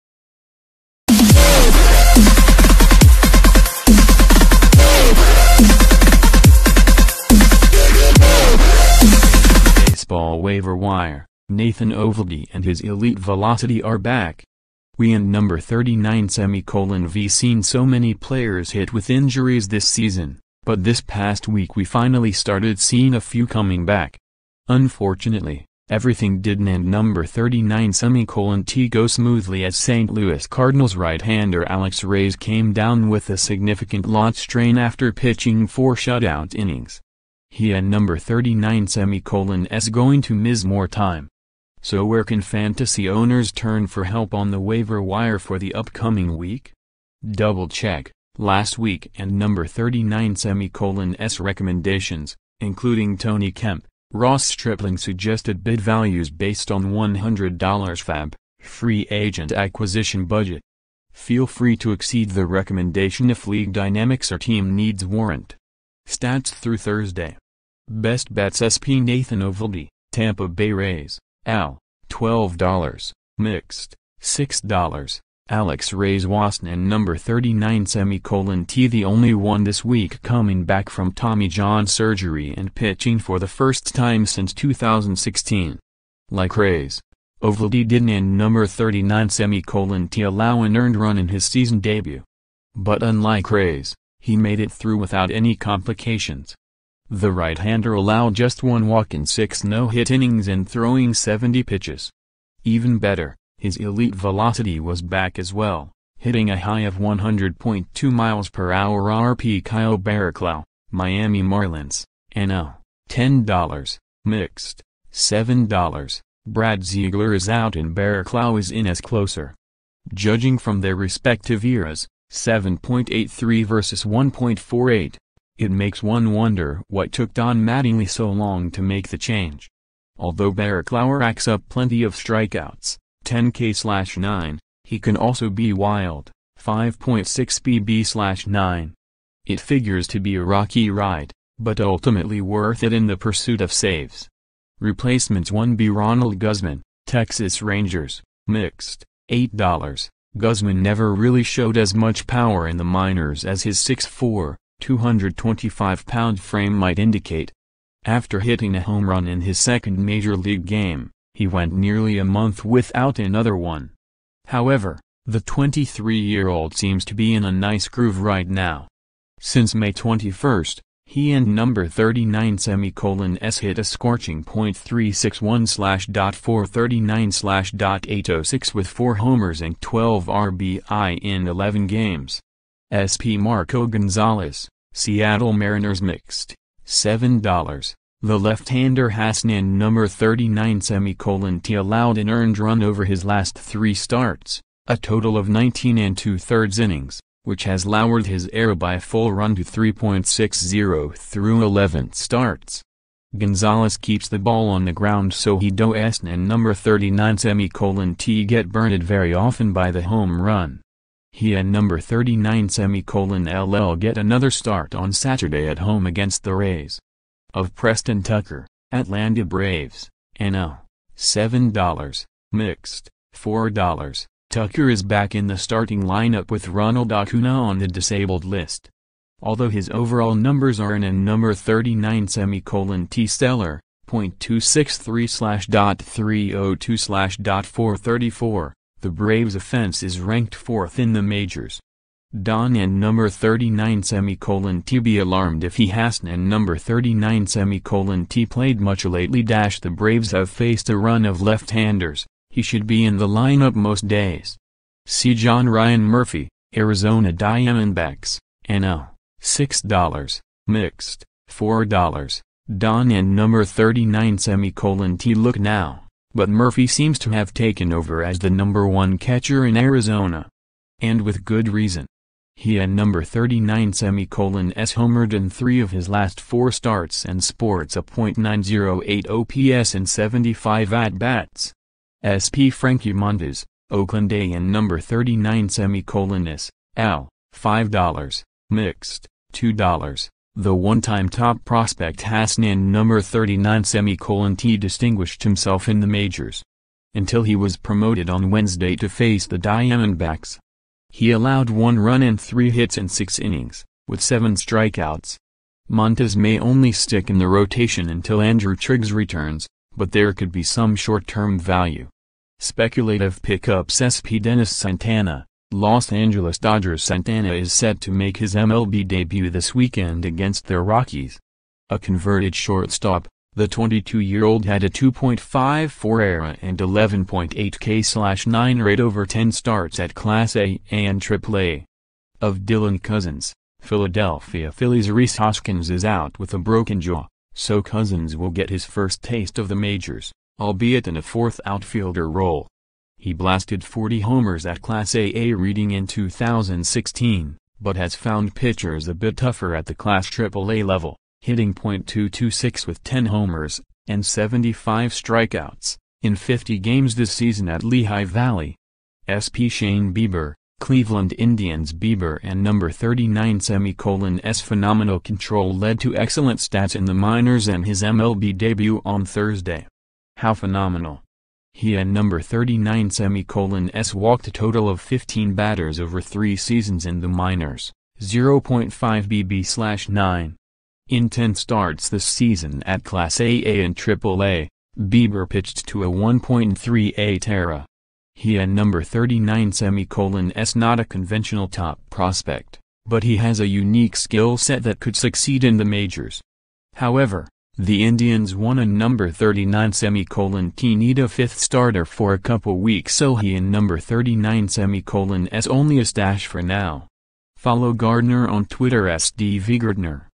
Fantasy baseball waiver wire Nathan Eovaldi and his elite velocity are back
Fantasy baseball waiver wire: Nathan Eovaldi and his elite velocity are back. We&#39;ve seen so many players hit with injuries this season, but this past week we finally started seeing a few coming back. Unfortunately, everything didn&#39;t go smoothly as St. Louis Cardinals right-hander Alex Reyes came down with a "significant" lat strain after pitching four shutout innings. He&#39;s going to miss more time.

So where can fantasy owners turn for help on the waiver wire for the